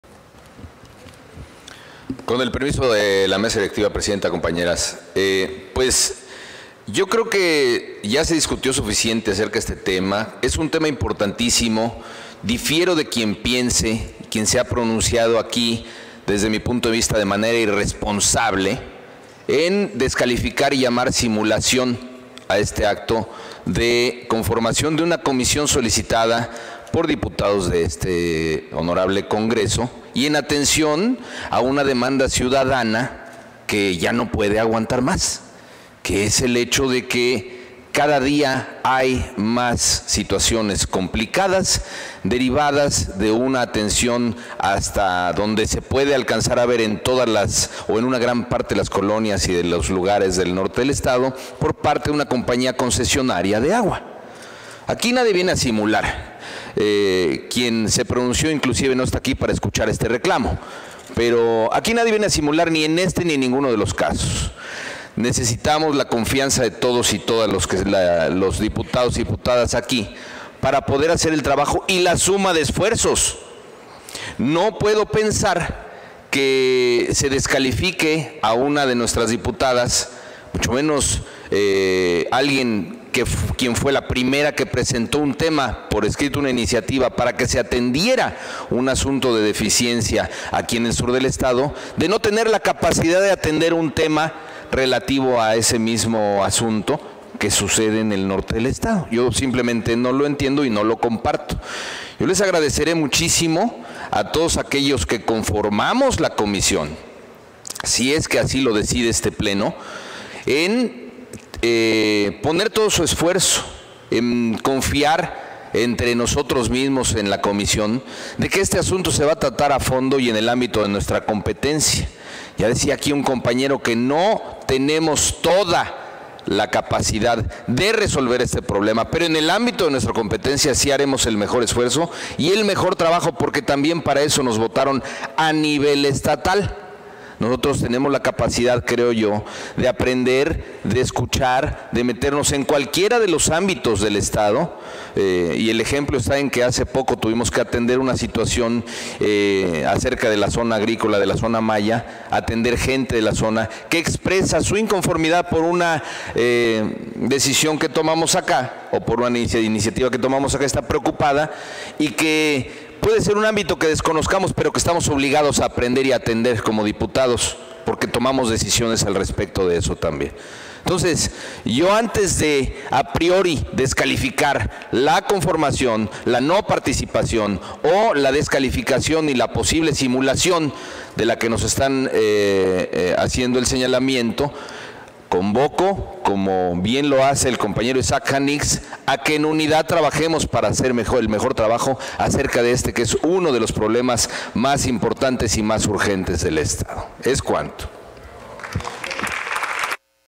Intervención Dip. Luis Humberto Aldana Navarro. MORENA (28/Marzo/2023)
Intervención del Dip. Luis Humberto Aldana Navarro, de MORENA, en relación al Acuerdo de la Junta de Gobierno y Coordinación Política de la H. XVII Legislatura del Estado Libre y Soberano de Quintana Roo, por el cual se aprueba la creación de la Comisión Transitoria Especial para el Seguimiento y Atención de los tema referentes a las concesiones otorgadas a la Compañía Desarrollos Hidráulicos de Cancún, S.A. de C.V. “AGUAKÁN” de la H. XVII Legislatura del Estado; presentada por los integrantes de la Junta de Gobierno y Coordinación Política de la Honorable XVII Legislatura; para su aprobación, en su caso. Publicado en la Gaceta Parlamentaria Año I, número 43 Ordinaria, de fecha 28 de marzo de 2023.